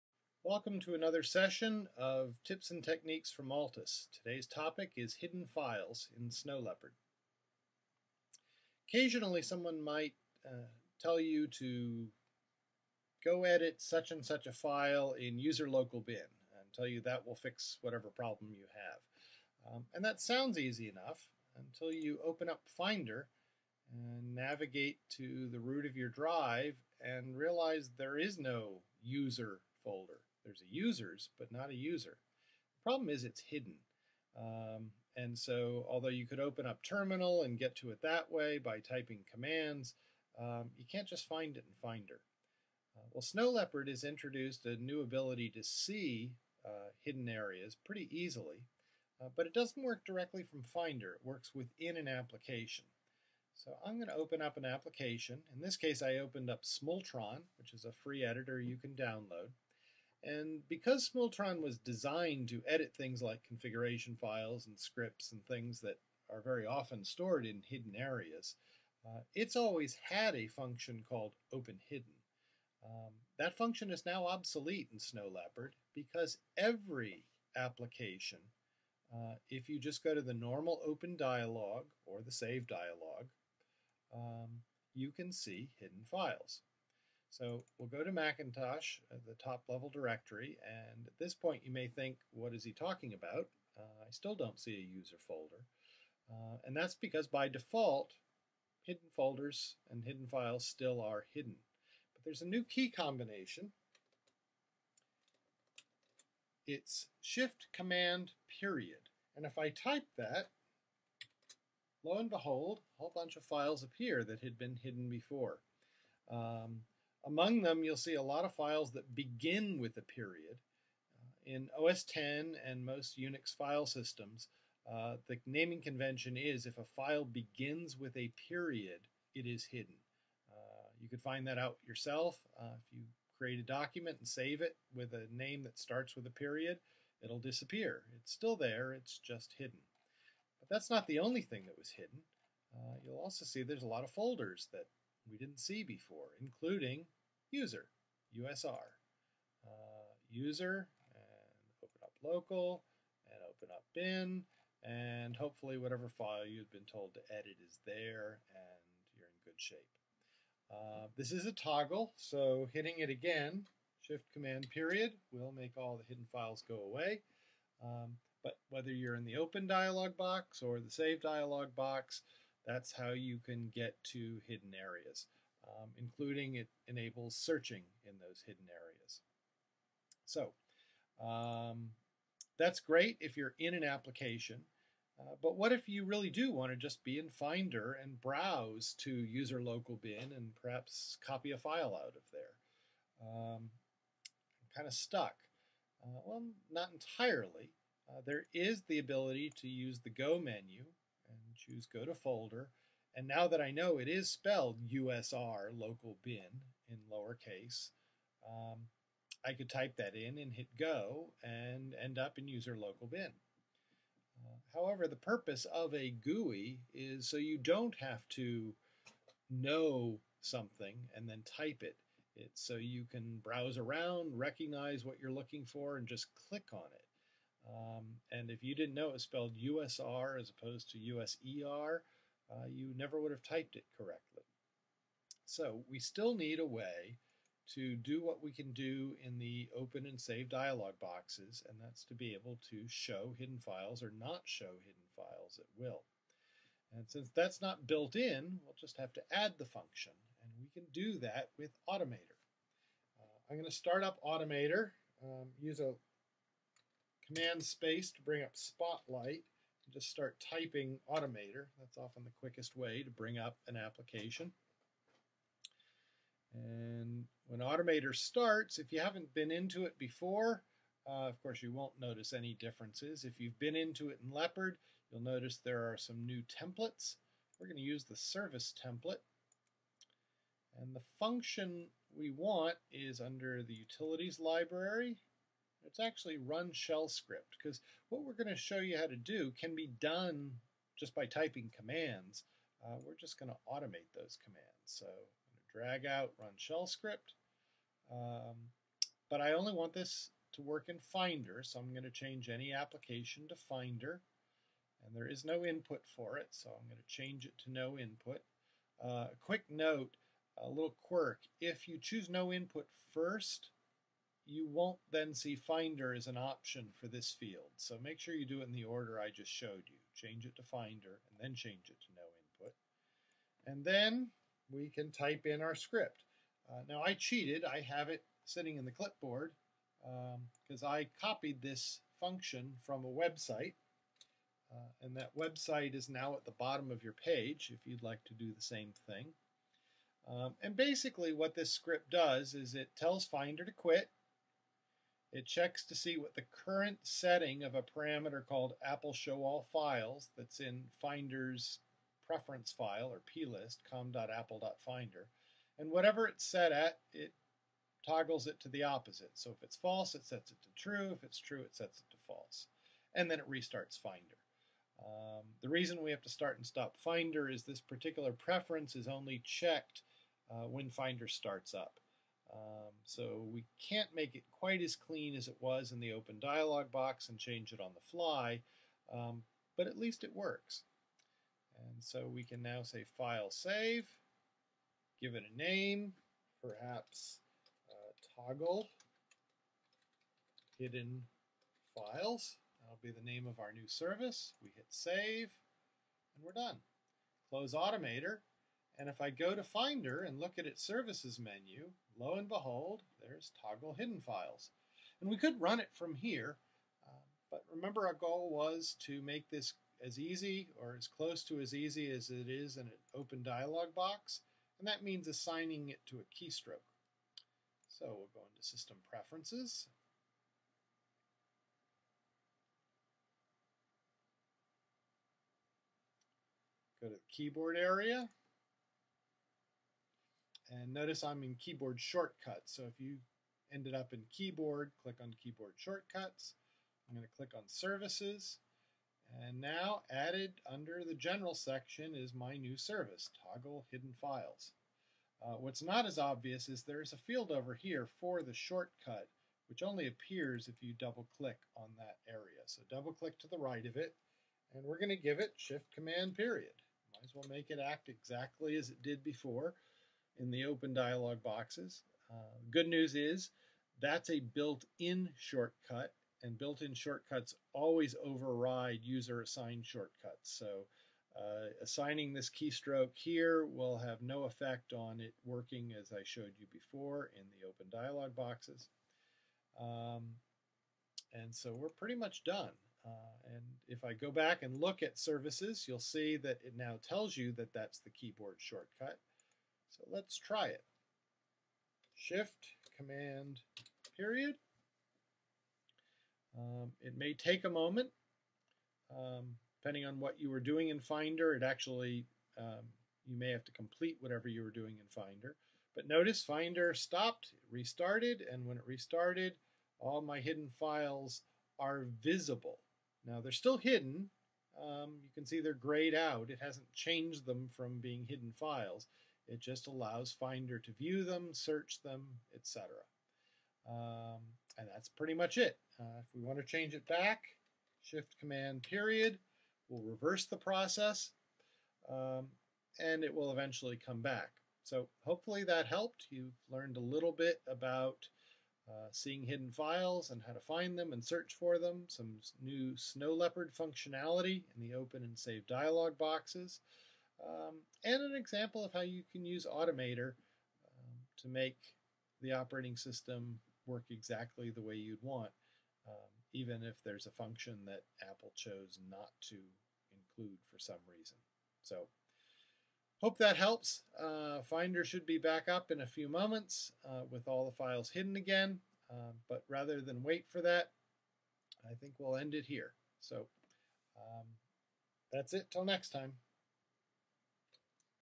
Tutorial: Hidden Files in Snow Leopard (OS X 10.6)
How to view/use hidden files in Snow Leopard